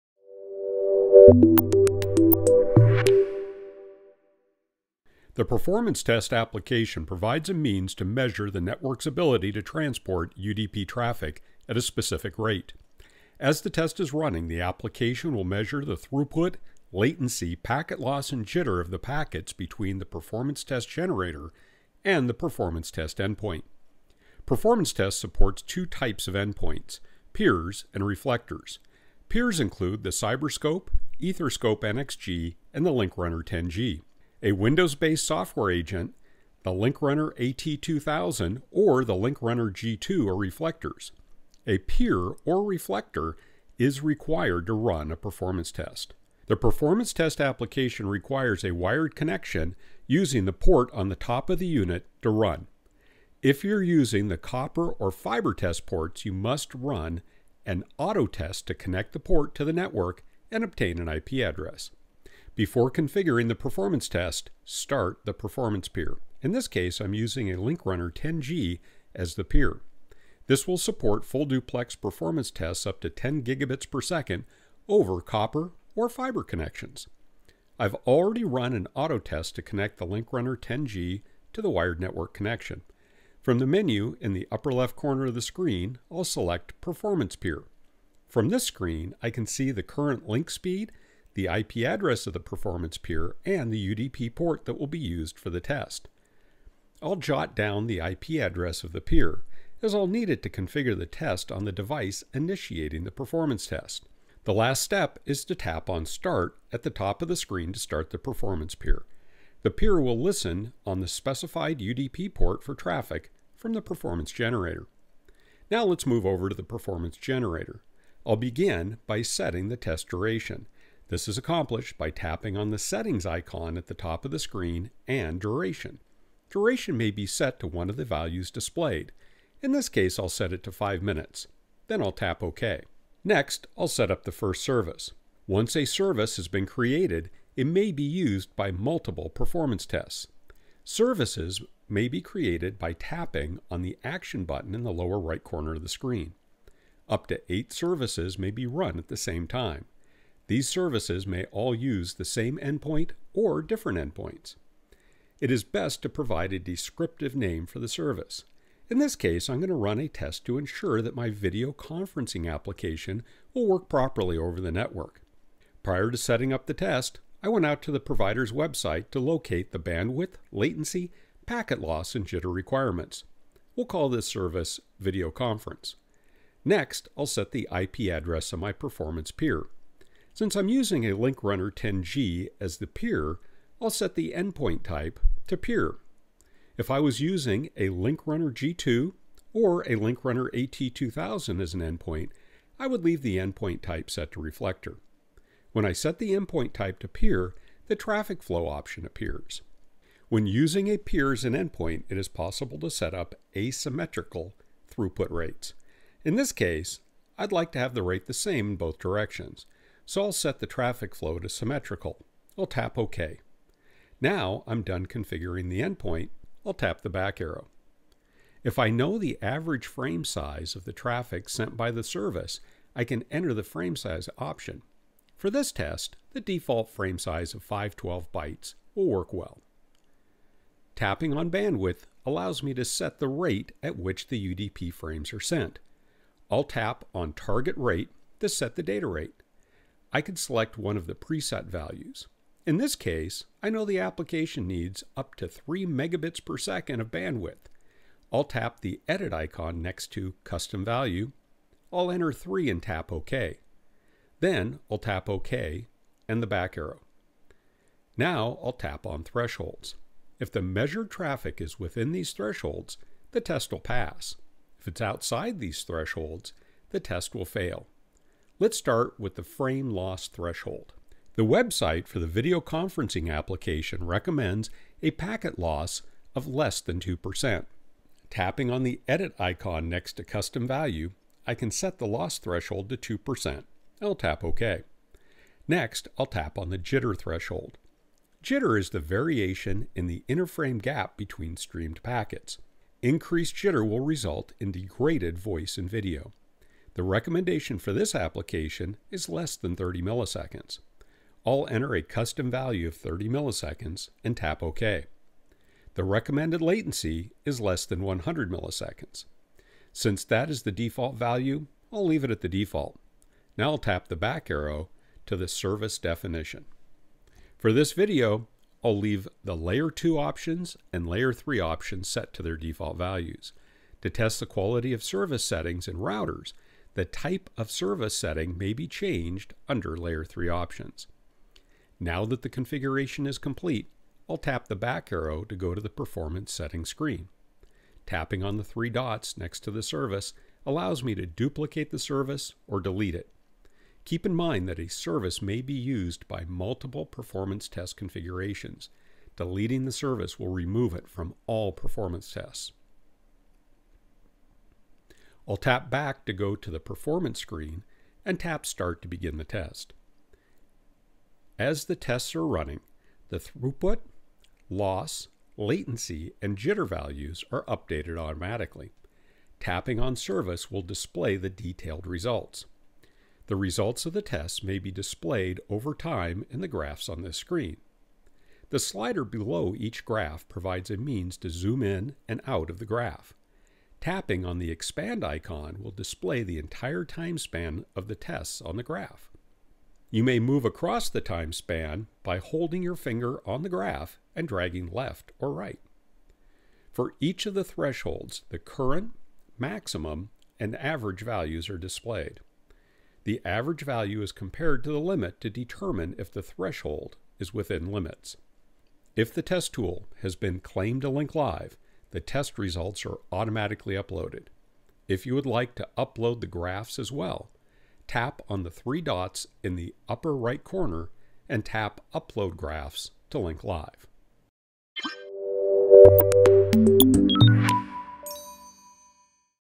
App Training - Performance Test
The 10G line rate Performance Test provides point-to-point performance testing of a traffic stream across wired IPv4 network infrastructure. This test quantifies network performance in terms of target rate, throughput, loss, latency, and jitter.